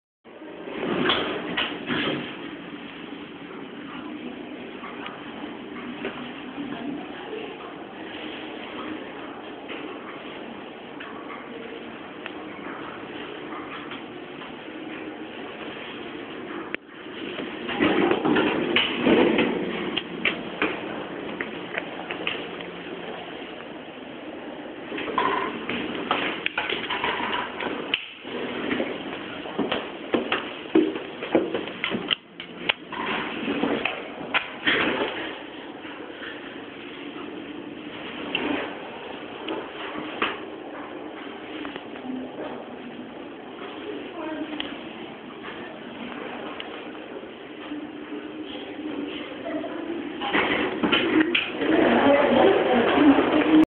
うわっくせーがお送りする 熊本 産交ビルのレトロな エレベーター.3gp
三菱
２機
かなりレトロな方だと思われる